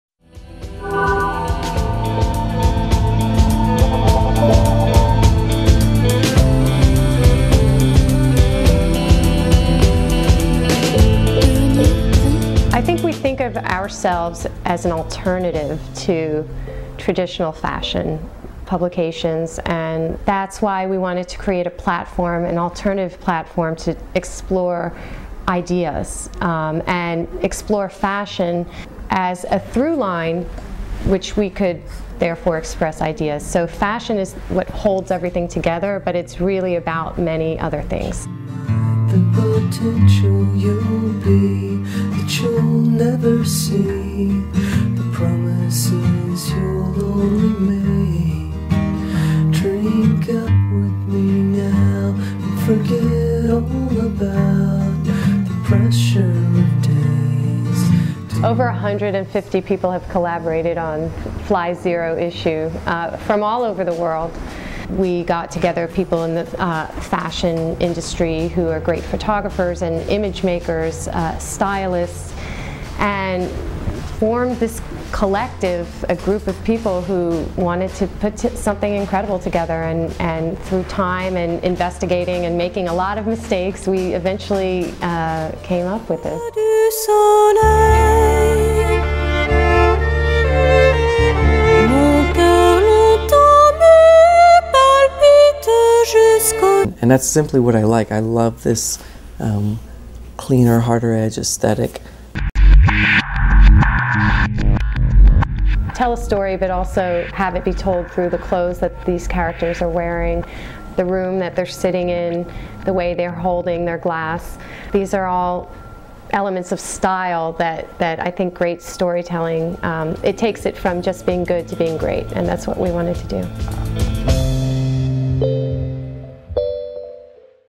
FLY Fashion DVD
An (exclusive) interview with Catherine Cushman, Creative Director of the new Fashion DVD FLY, who talks with Cool Hunting about the ideas and people behind making the ambitious project happen. Includes clips that feature design by Marc Jacobs, Zach Posen, Valentino and an interview with Ryan Mcguiness.